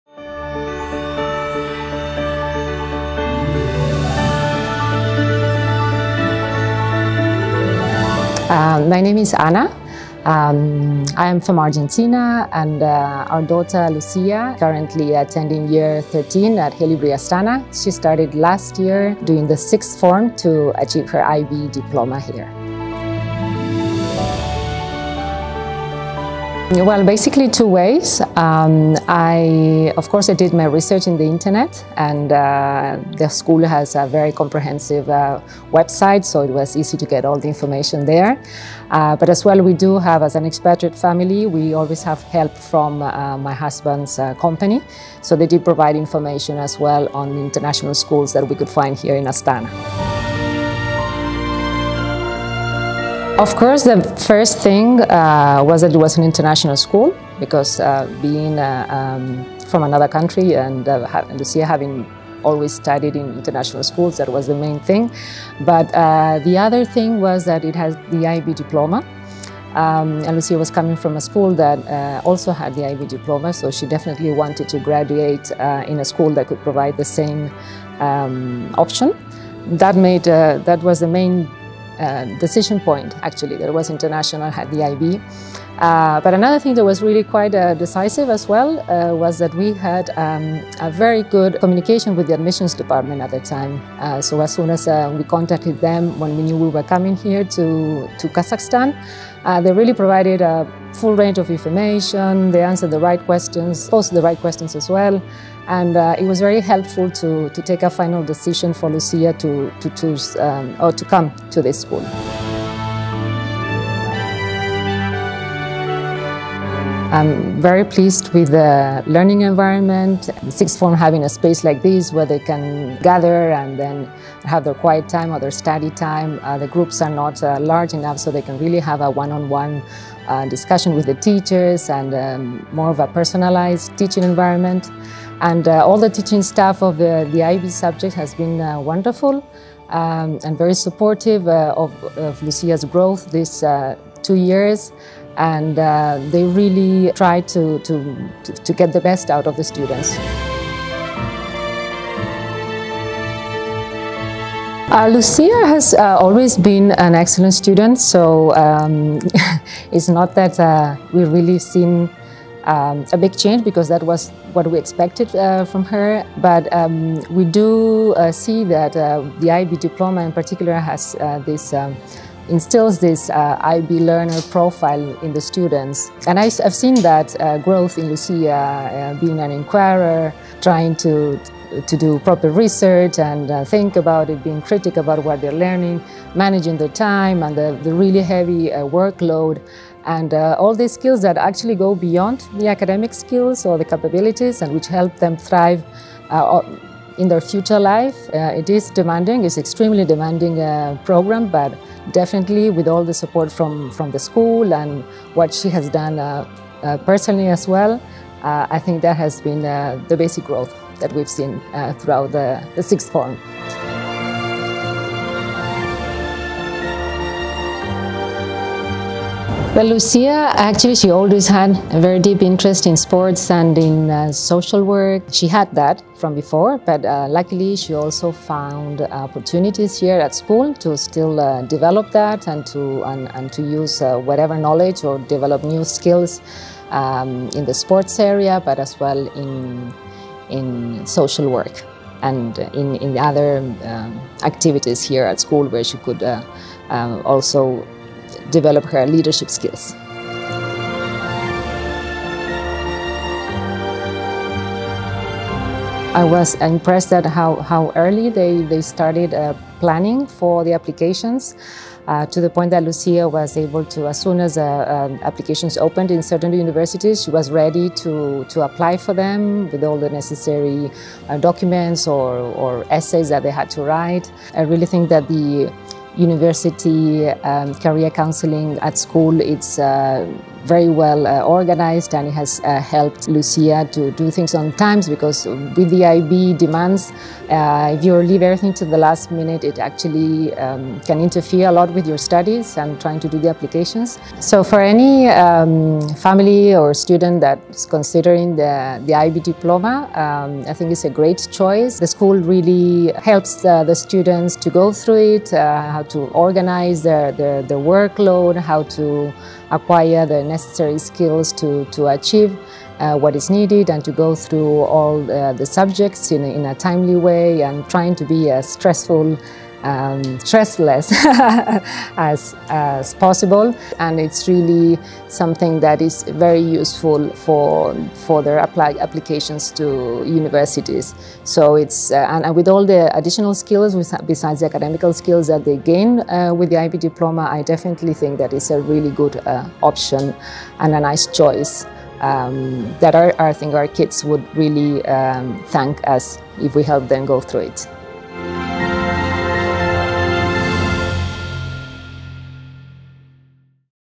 Parents' Feedback / IB Diploma Programme
Behind every pupil at school are parents who have taken great responsibility in choosing a school for their child. In this video, Ana Burmeister, mother of Lucia, shares her experience of picking a school while being abroad and her opinion about the school’s facilities and the admissions work at Haileybury Astana.

Lucia is a bright Year 13 pupil who studies the IB Diploma Programme and has performed exceptionally well academically and athletically over the past two years. She has also received University offers later on, which we are going to share with you in the near future. For now, we invite you to watch her journey at Haileybury Astana.

If you would like to learn more about the admissions process, please or contact our Admissions department at .